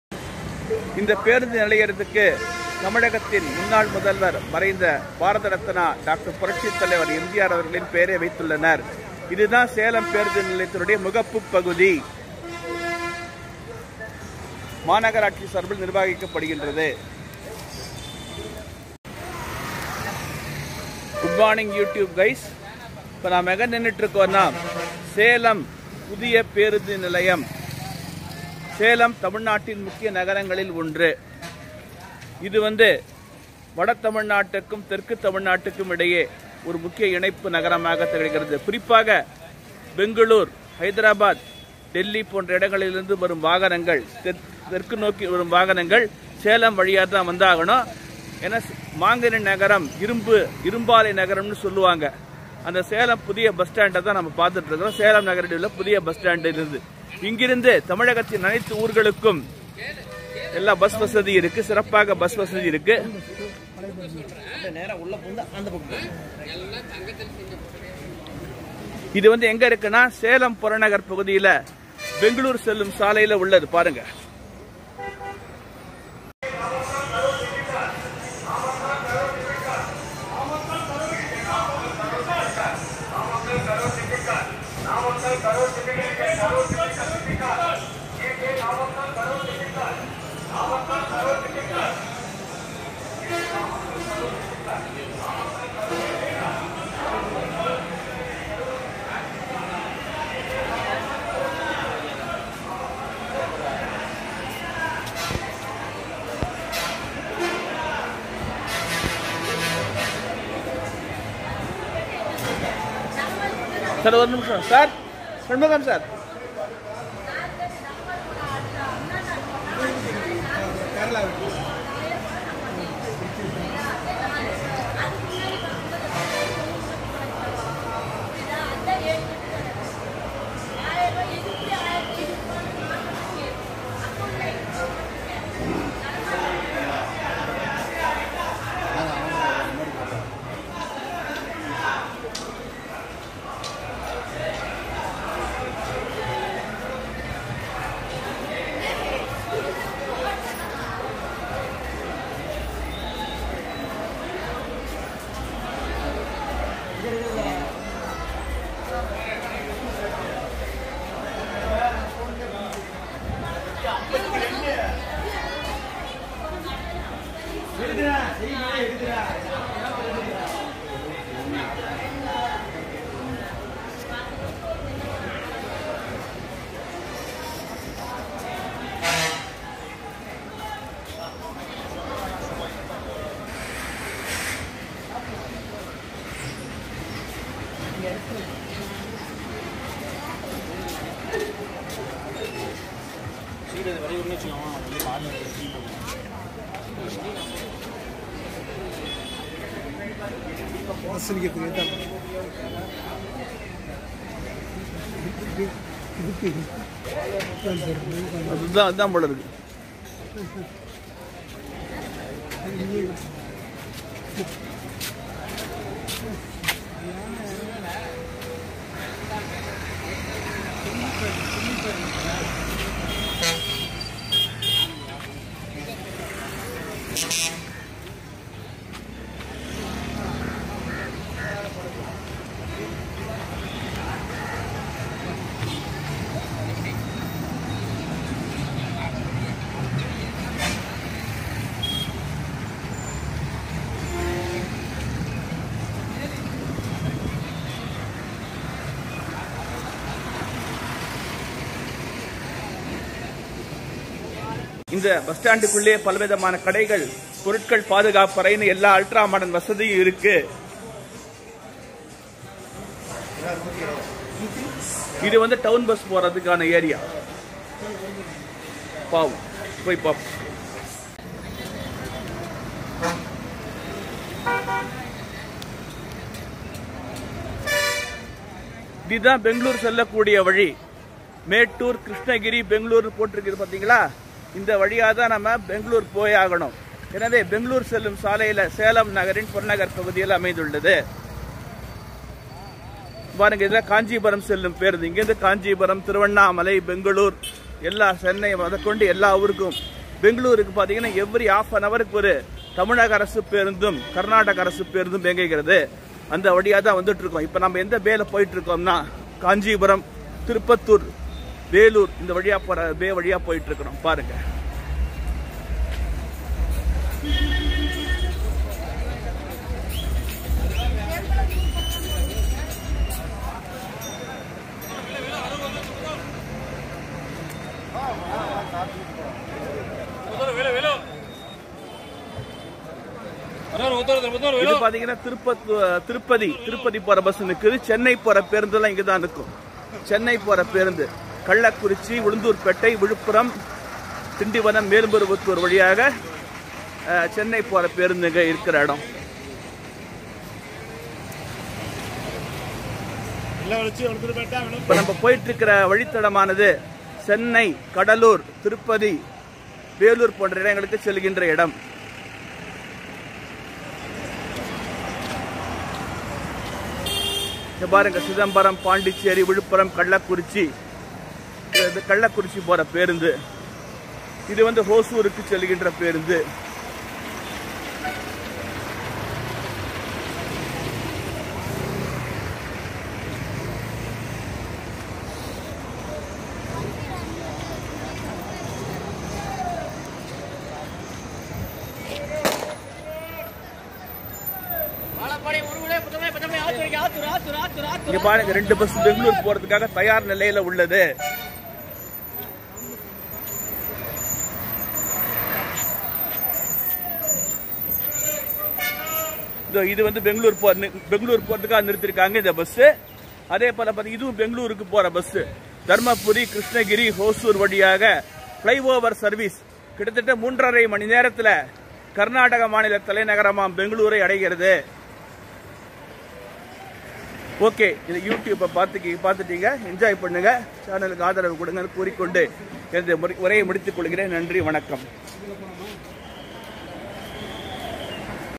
VLOG ON SALEM NEW BUS STAND
SALEM CORPORATION's  DR.M.G.R NEW BUS STAND LOCATED AWAY   FROM TOWN LOCATED AT MEYYANUR LOCATED AT THE OUTSKIRTS OF CITY IN PROXIMITY TO BYE PASS ROAD TOWARDS BENGALURU . WATCH THIS  VIDEO COMPLETELY AND SUGGEST YOUR VALUABLE COMMENTS